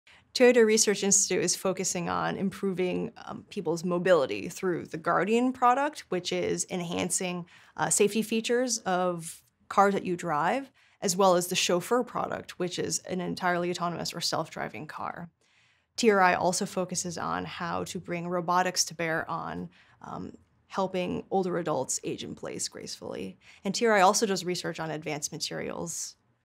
Toyota Research Institute Guardian Product | Robotics Manager Tiffany Chen | Career Girls
Watch Tiffany Chen, Manager of Human-Machine Interaction Research at Toyota Research Institute. She leads a team of researchers working to improve the performance and safety of self-driving cars.